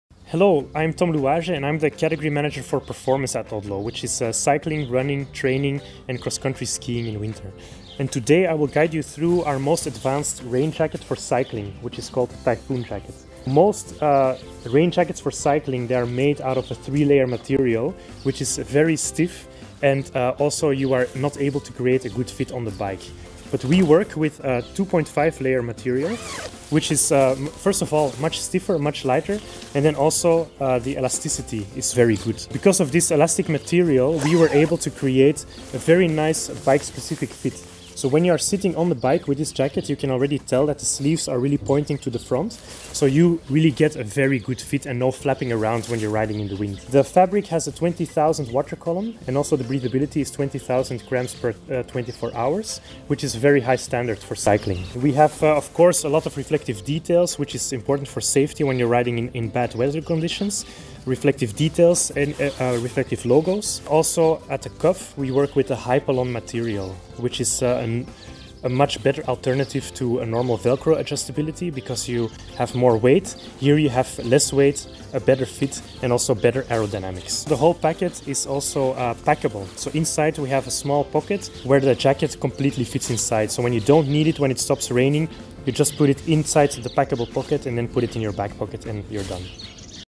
Typhoon Women Jacket - Performance Biking & Cycling
High performance, Aerodynamic, full-stretch rain jacket designed for cycling.
Made from three-layer, four-way stretch fabric, the men’s Tyfoon Jacket will mould itself to you when you are cycling, creating an aerodynamic body profile that moves with you and reduces ‘flapping’ as you eat the kilometres. Odlo’s logic fabric technology is included in this performance, lightweight rain jacket’s construction and this is 100% waterproof to ward off rain showers as well as windproof and highly breathable. A great body fit, complete with a long back increases the sense of protection, whilst for ease of use the Tyfoon Jacket is packable in a handy removable pouch and includes reflective details for safety. Please take note that this jacket has been designed to offer a perfect fit when sitting on the bike. Therefore while standing, the fit might feel unusual.
Connect to Odlo world to discover all the best Sportswear Performance Clothing: odlo.com